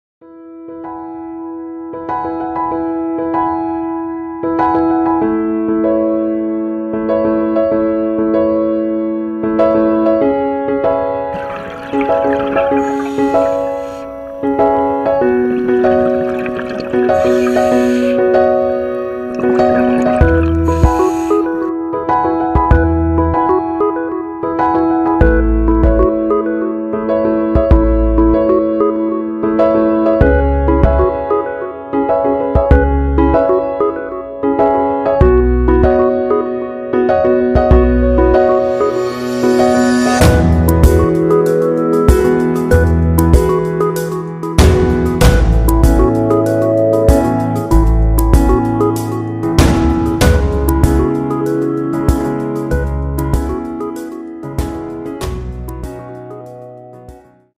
Sea Project for filmmaking
This is a short film about filmmaking about the red sea this includes the loggerhead turtle, angel fish and some divers in Ras Mohammed